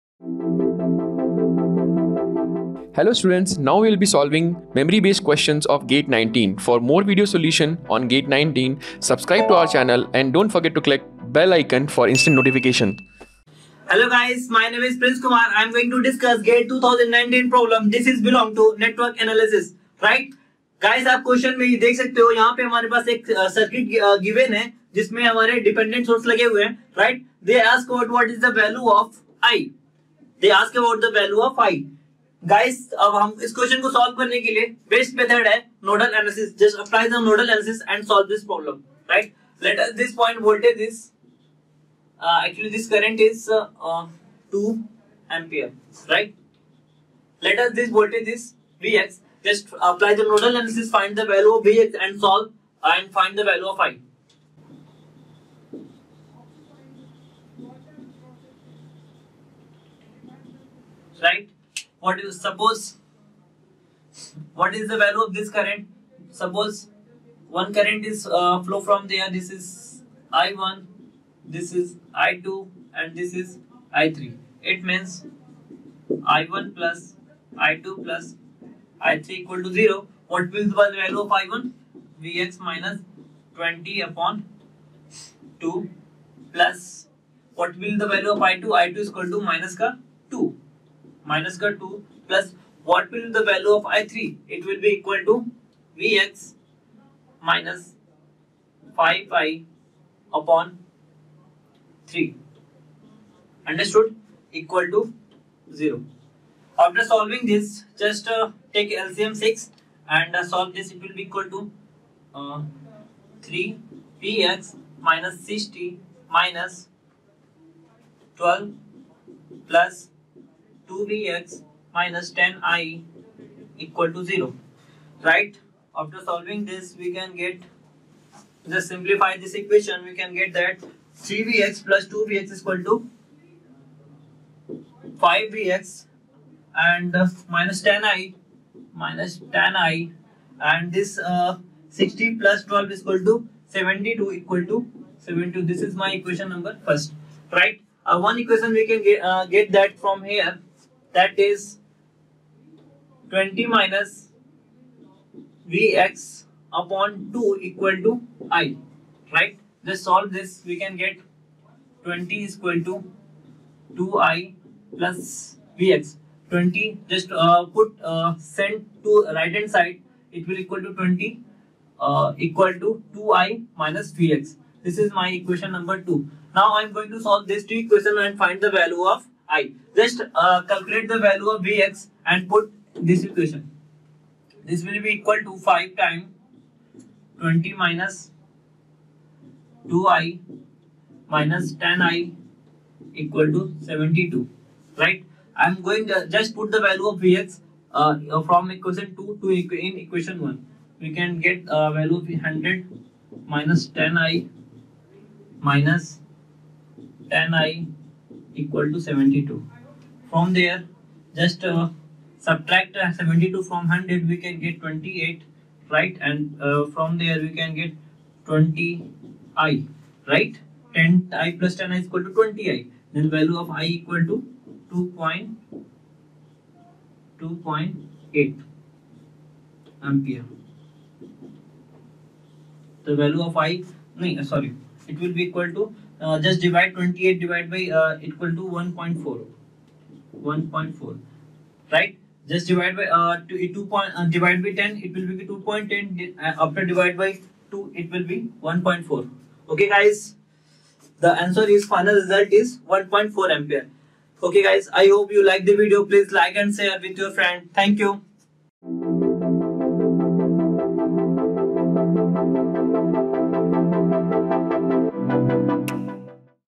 GATE 2019 Answer Key - Video Solution for Electrical Engineering | Network Theory - 03
GATE Paper Analysis Electrical Engineering 2019: GATE exam analysis for GATE Electrical Engineering paper with Answer Key and detailed video solutions.

Do not forget to download GATE 2019 Answer Key for EE with Question Paper and Video Solutions.